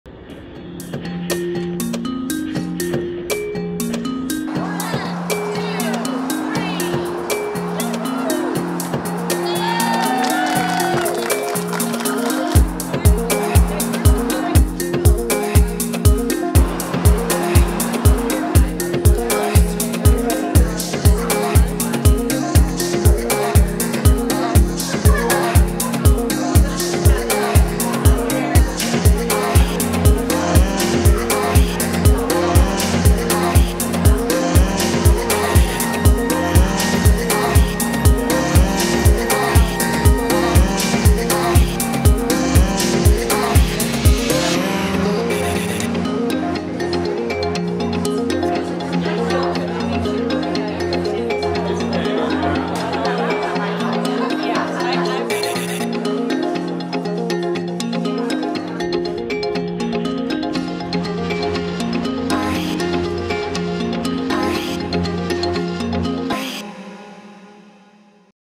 Tulipe Ribbon Cutting
Tulipe Lifestyle store celebrates their ribbon cutting and grand opening in Richland